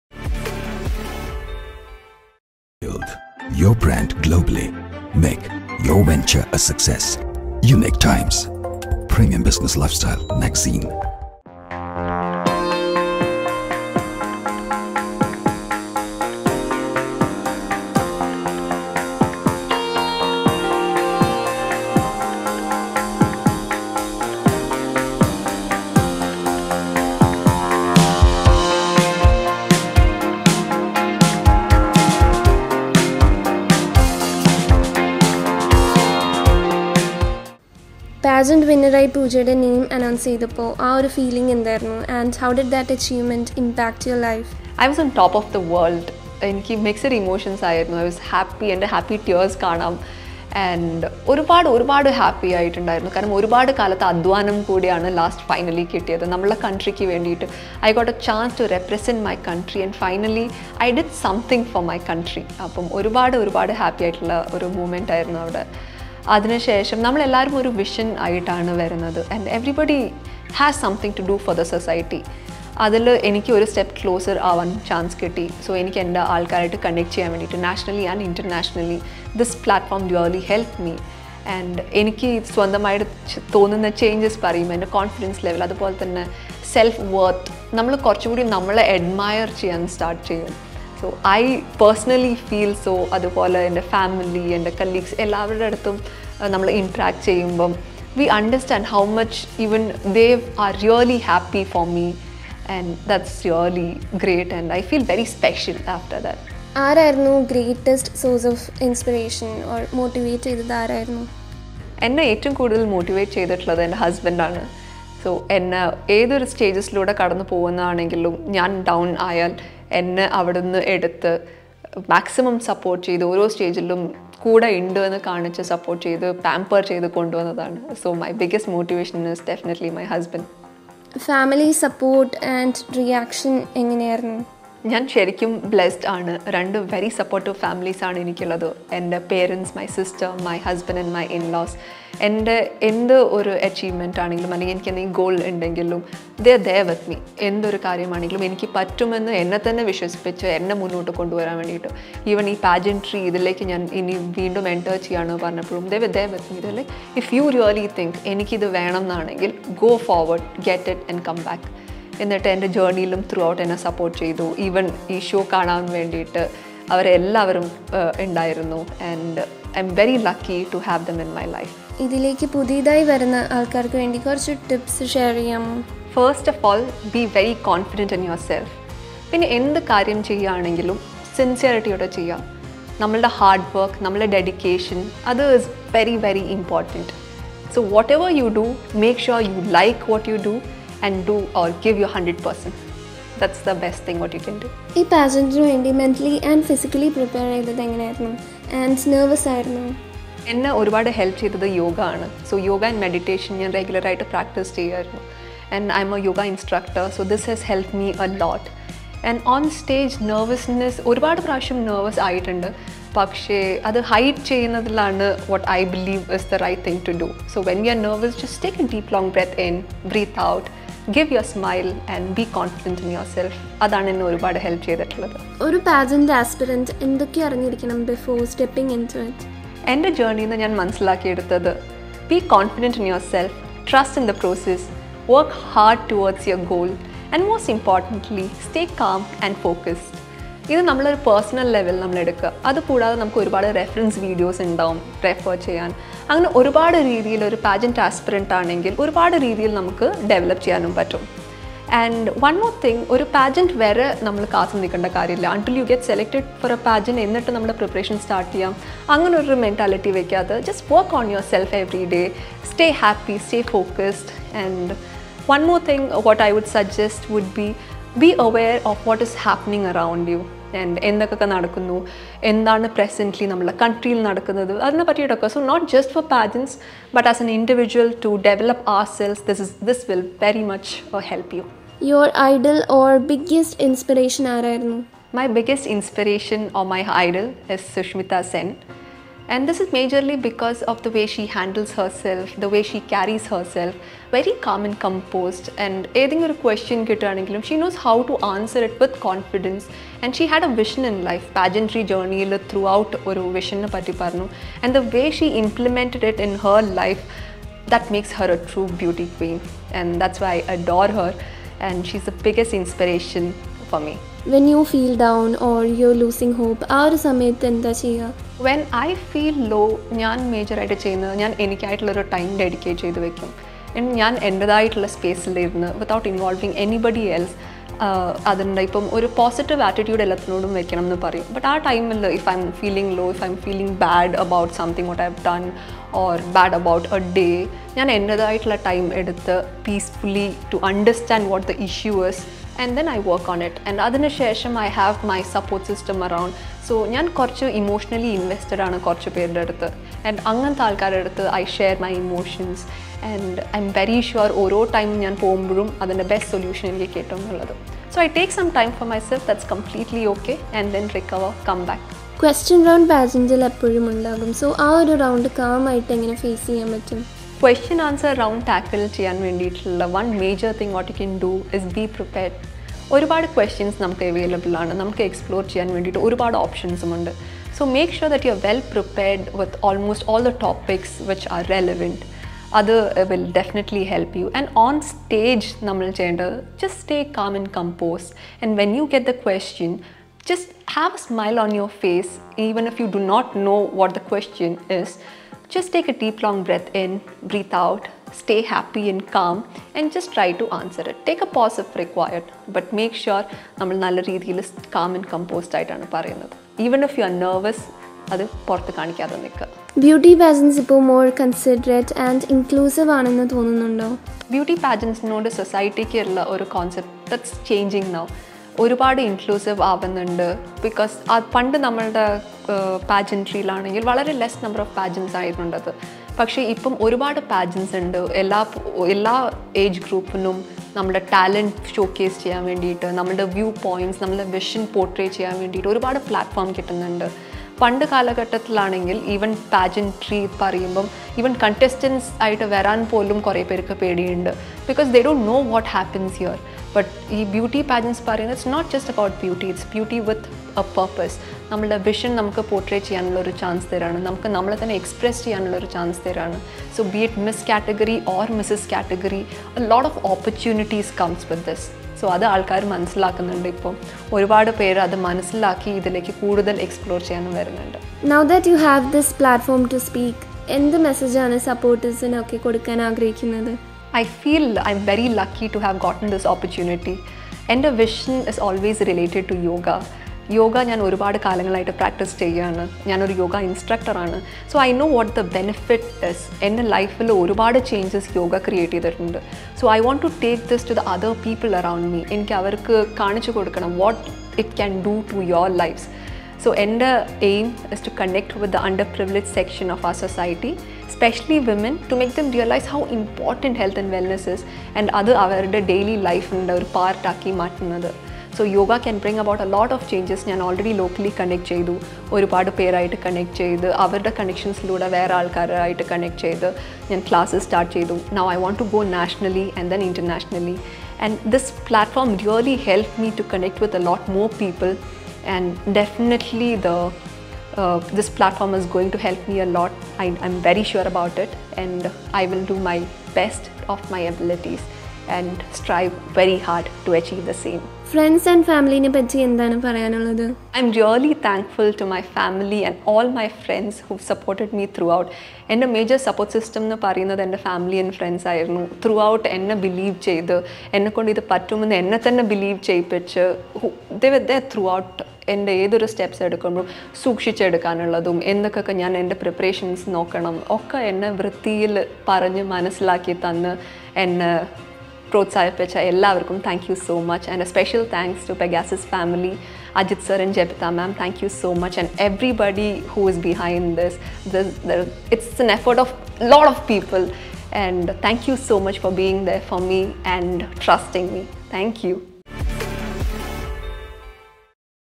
POOJA MOHAN | MRS GLAM WORLD - 2023 - WINNER | INTERVIEW | UNIQUE TIMES MALAYALAM | PART-2
MRS GLAM WORLD - 2023 - WINNER | POOJA MOHAN | INTERVIEW | UNIQUE TIMES MALAYALAM | PART-2 |

An event by: @pegasusglobalpvt
Event directed by : @drajitravi

Mrs Glam World 2023 - Mrs India 🇮🇳 @pooja.mohan
1st Runner Up -  Mrs Russia 🇷🇺@bocharovadarya0708
2nd Runner Up -  Mrs Malaysia 🇲🇾 @niran_nraa